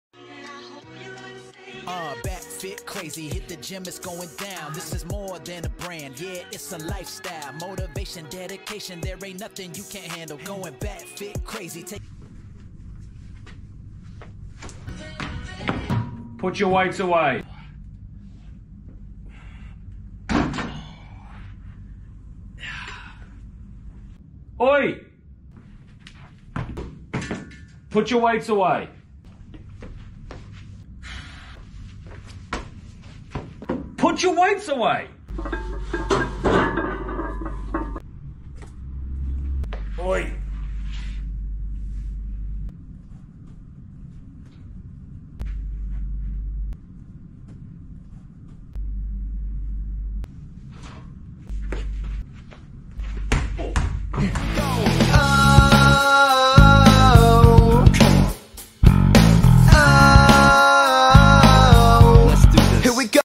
Please put your weights away
A much needed part 2!
Please guys, have the courtesy to put your weights away. Some people haven’t been able to use equipment they want because they aren’t able to lift the weights!